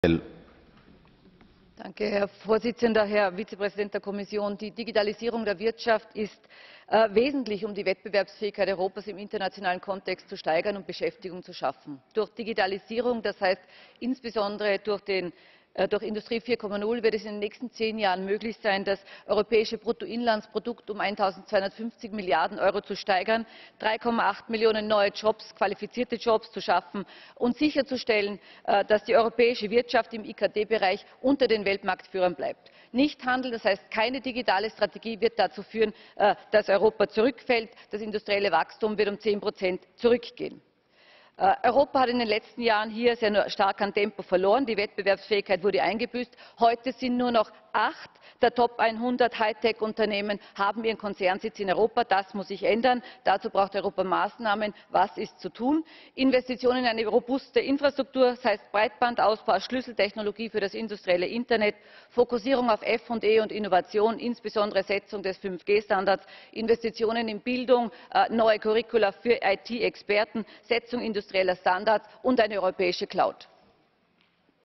Barbara Kappel - Industrie 4.0/Digitale Strategie (Plenarrede im Europäischen Parlament)
Plenartagung vom 19/05/15, Beschluss über den digitalen Binnenmarkt
Thema: Digitalisierung der Wirtschaft um die Wettbewerbsfähigkeit von Unternehmen zu steigern und neue Arbeitsplätze zu schaffen, Industrie 4.0, Breitbandausbau, Big Data